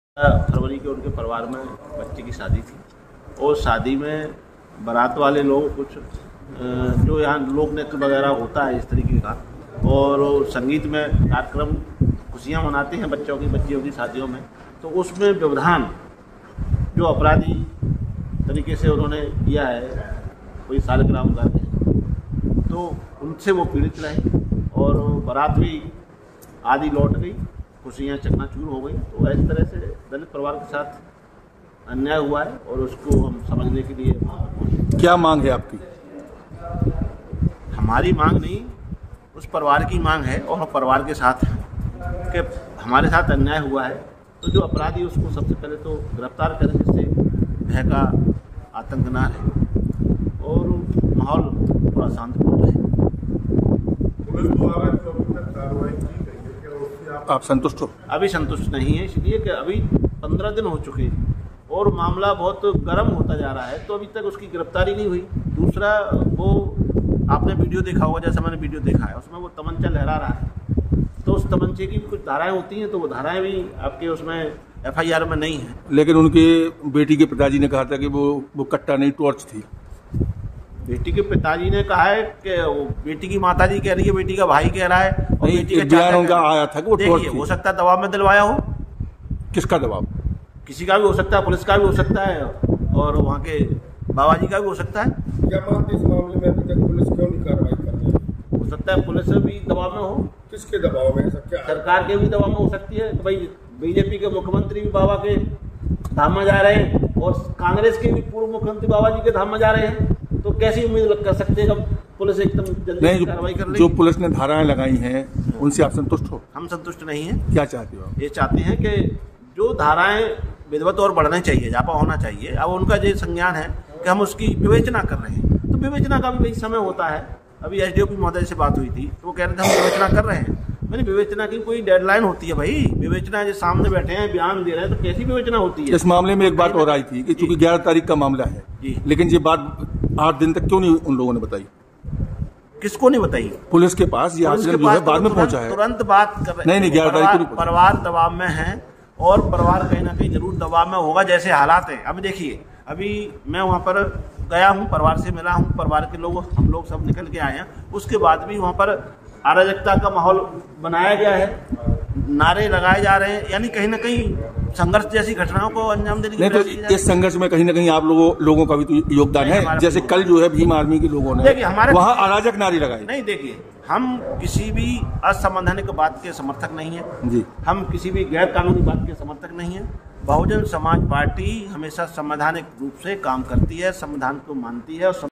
बागेश्वर बाबा के गांव से अब बसपा ने कर दिया बड़ा ऐलान || BSP | Mayawati | Bageshwar Dham
हमसे जुड़े -